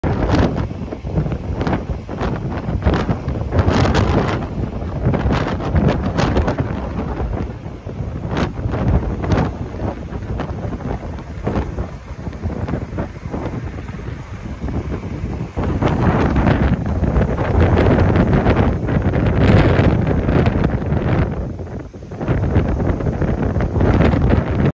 Video: State Road A1A closed at Summer Haven due to overwash
Thank you to Walter Coker for the video of flooding on A1A.